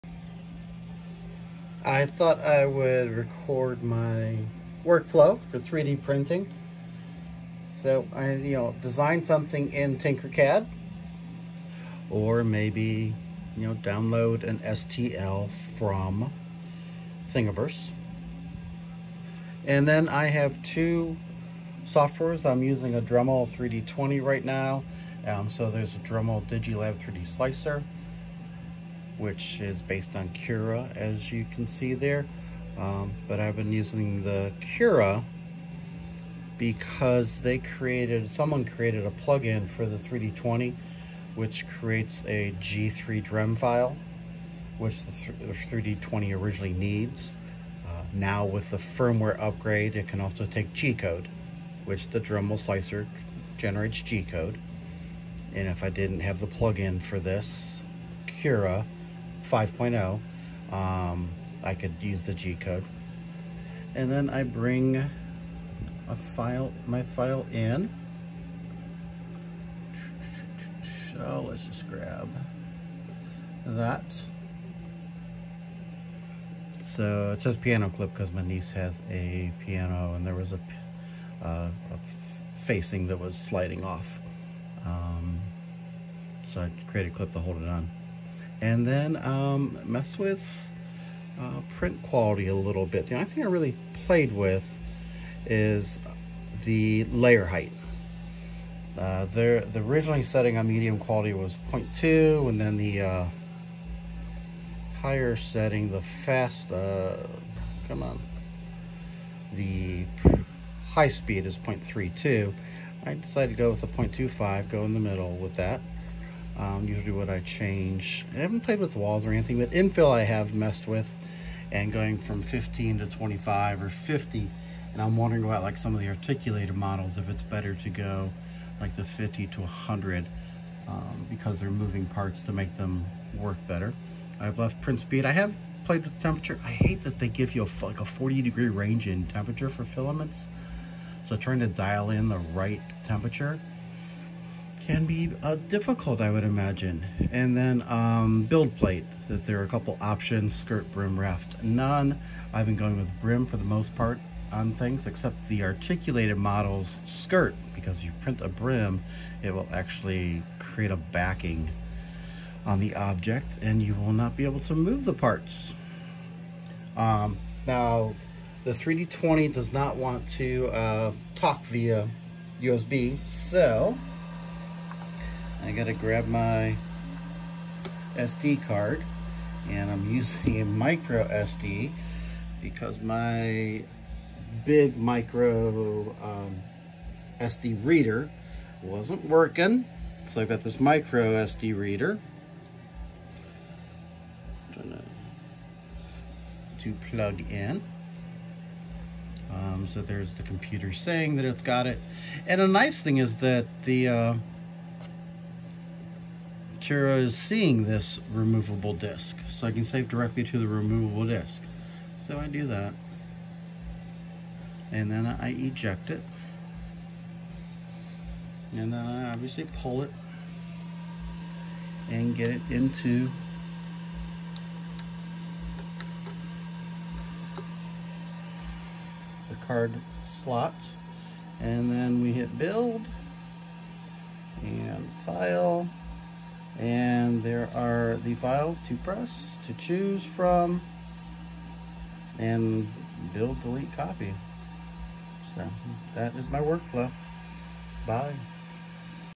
3DprintWorkflow
sharing my workflow for 3D printing
design in Tinkercad or download an STL form somewhere like Thingiverse
open Cura or Dremel Slicer
open the STL
Maybe adjust some print settings (like layer height or infill or brim/skirt)
get the .gcode or .g3drem (original file type the Dremel 3D20 used - after firmware update it can take .gcode also) file to SD card
put SD card in printer and build